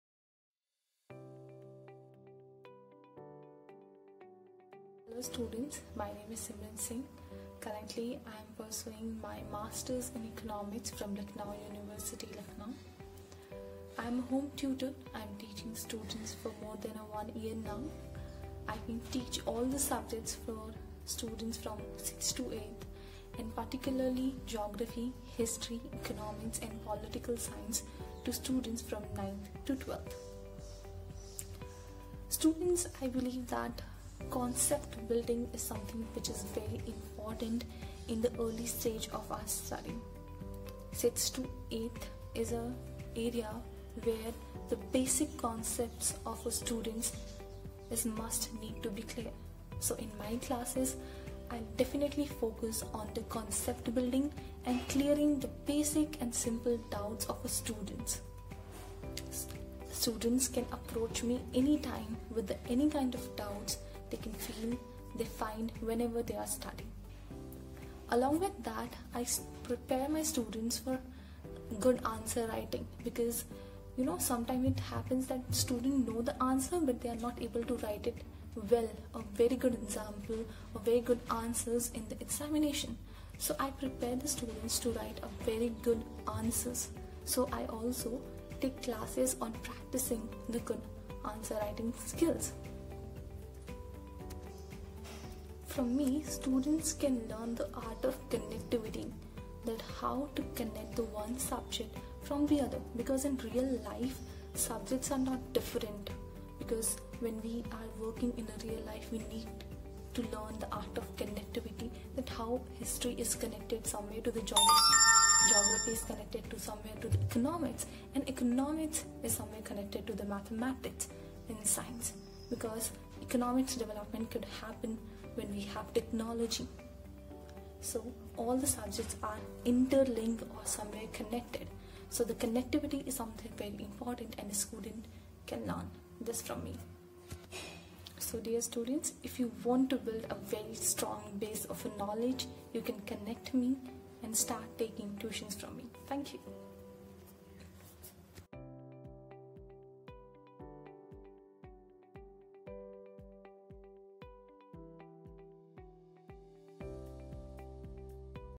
Home Tutor in Golf City, Lucknow for Class 8 Tuition - Simran Singh
i am home tutor for more than a year now. i can teach all subjects to students 6 to 8 and history geography economics and political science to students of class 9 to 10 . i have completed my bachelor's degree in 2023( political science English and economics) . This year i will persure my MA economics course. my skills are communication, answer writing skills , Better understanding of concepts. along with building conceptual base of students i will also trained them to write a good answer for the exams .